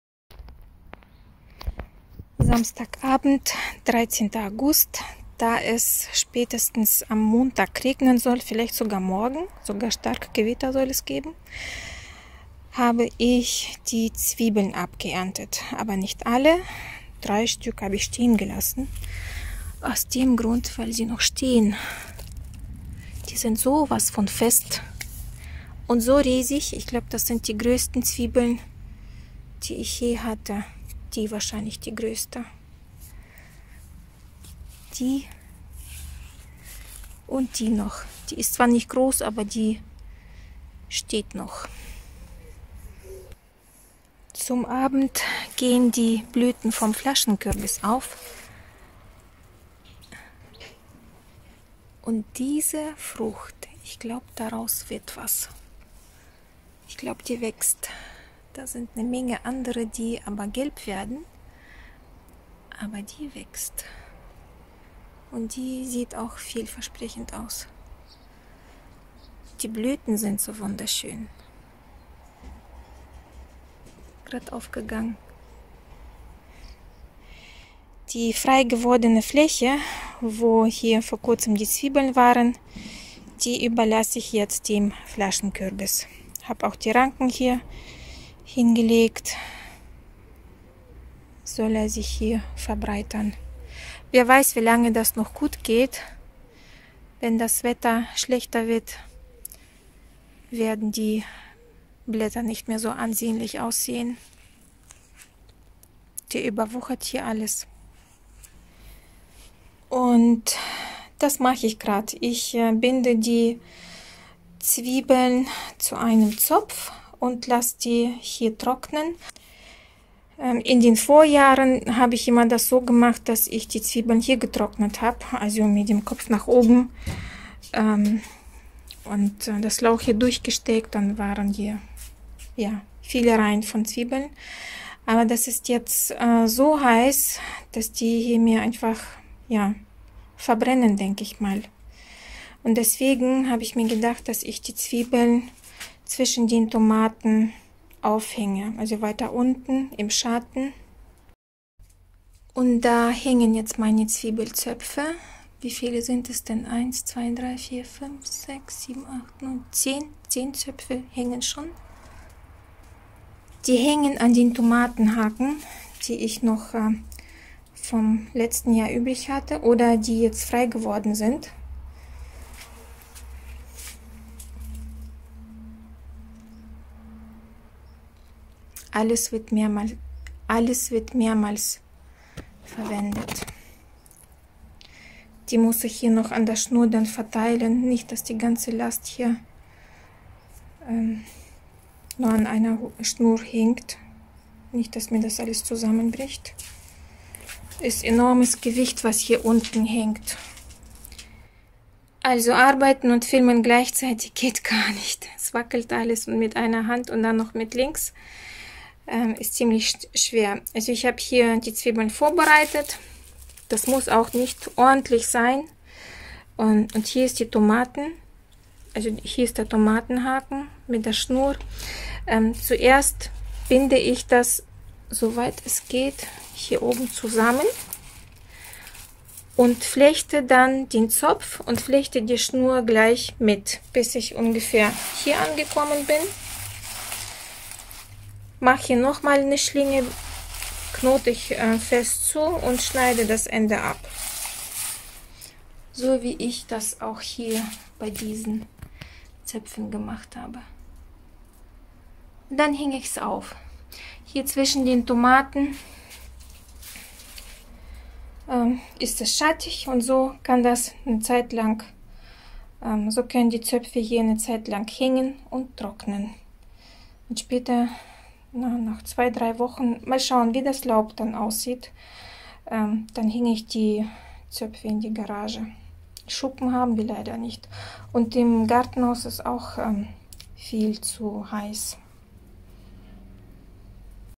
Zwiebeln zu einem Zopf binden und zum Trocknen aufhängen
Meine aus Saatgut gezogene Zwiebeln sind reif und wurden abgeerntet.
Das sind Sorten: Gemüsezwiebel Exhibition und Bodenseezwiebel  Höri-Bülle.
Aus Saatgut gezogene Zwiebeln haben einen großen Vorteil: sie möchten nur groß werden und blühen nicht, das Ausbrechen der Blüten fällt also aus, was sehr angenehm ist.
Wenn das Laub der Zwiebeln fällt und liegt, und der "Hals" dünn ist, ist die Zwiebel reif und kann aus dem Boden gezogen werden. Ist es soweit, sollte die Zwiebel nicht mehr nass werden. Nun war Regen angesagt worden und so mussten die Zwiebeln raus.